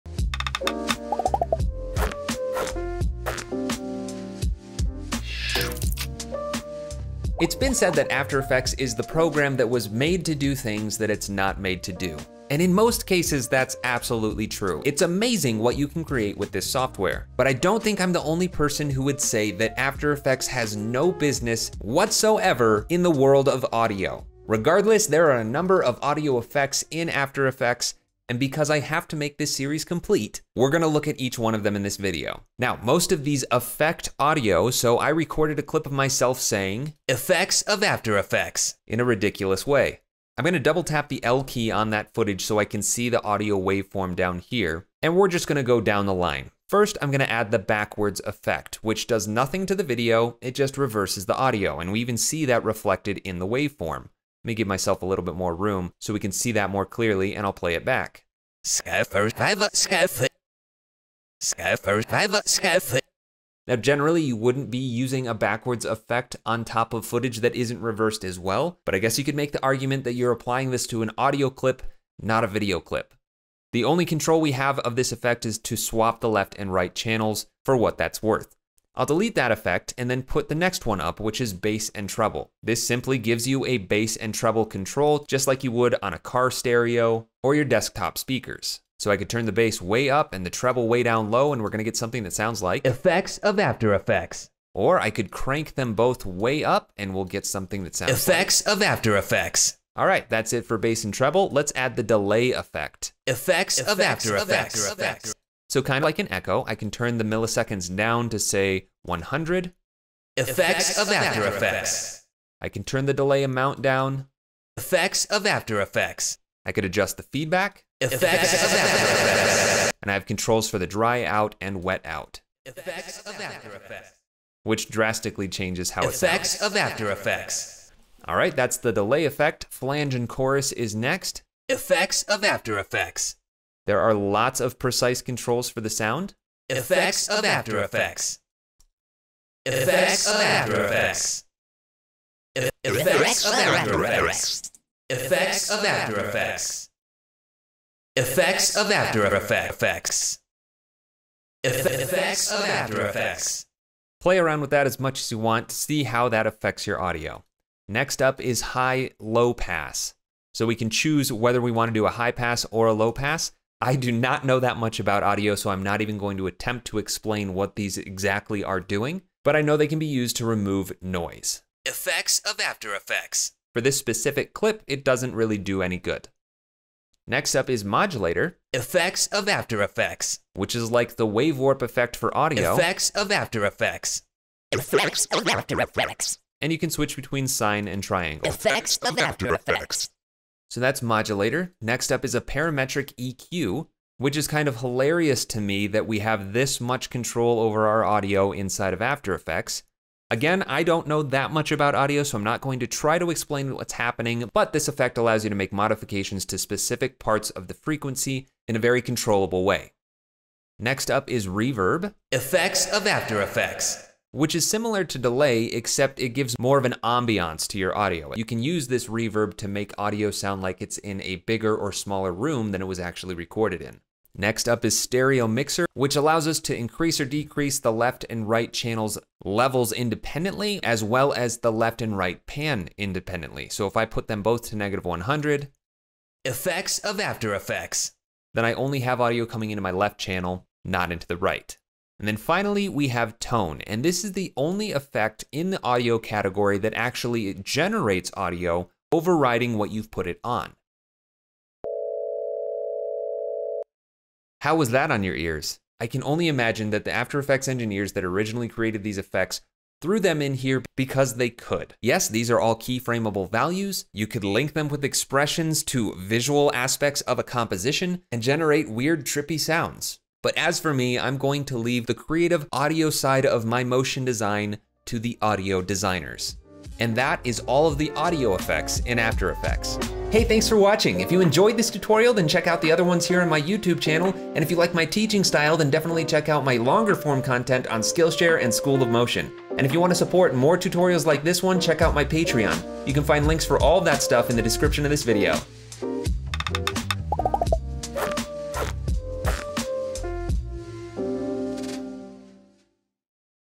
Audio | Effects of After Effects
In this quick tutorial, I explain how to use all of the audio effects in Adobe After Effects, and why you shouldn't.

Chapters:
00:00 Intro
00:46 Backwards
01:19 Bass & Treble
01:41 Delay
02:19 Flange & Chorus
02:38 High-Low Pass
02:57 Modulator
03:11 Parametric EQ
03:31 Reverb
03:44 Stereo Mixer
04:02 Tone
04:40 Thanks for watching!